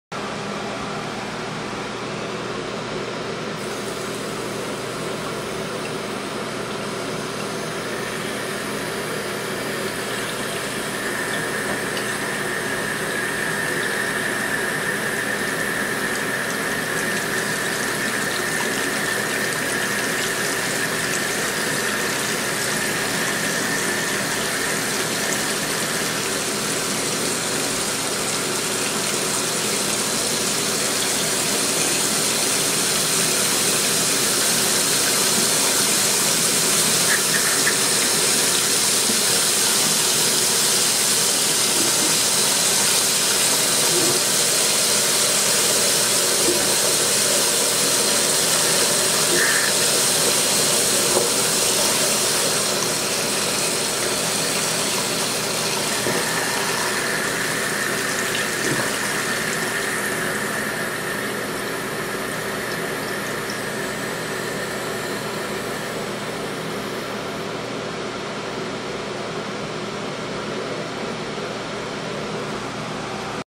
Thirsty for tap water? 4K
My sink is not dirty it has character. 29.95 FPS 4000th of a second 5.6f Canon EOS M with magic lantern 2x bitrate movie crop on and cine style profile, Also my 18K lumen light hehe. And Adobe After FX.